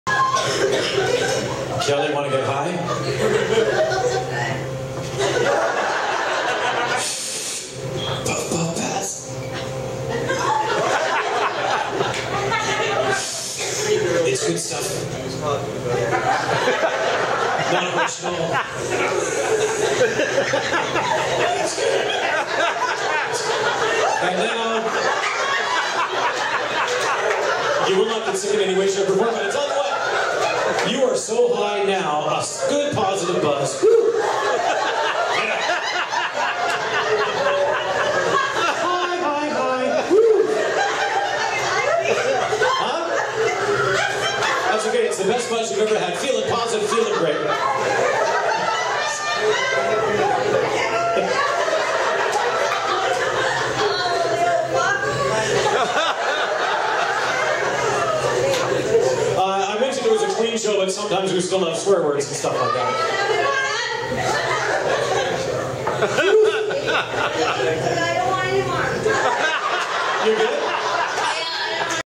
Kelly from Ice Pilots gets Hypnotized and really high
Kelly is such a great sport.  Such an honor to have one of Buffalo Air's famous staff in my show in Yellowknife